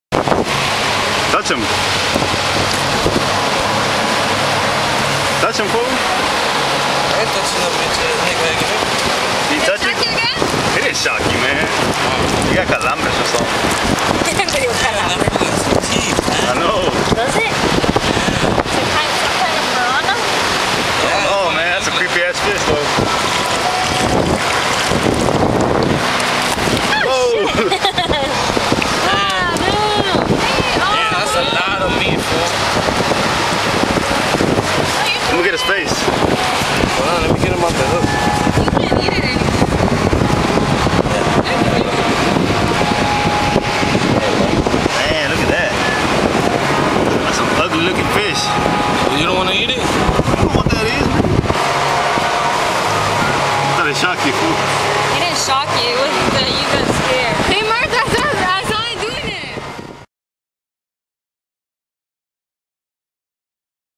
Told me it was a Dogfish(Officially a Star Gazer)
I guess it is a Dogfish.  That's what I was told.   It was hard to find any pictures of it on the internet to confirm it.  My friend said it shocked him.  But some one told us that fish was poisonous, and that shock feeling he was getting was the fish extracting poison.  Defense mechanism i guess.
Either way, its weird looking.  With its sharp teeth above its head and all.  Kinda looked like a bulldog. lol
Fished in Freeport, Tx.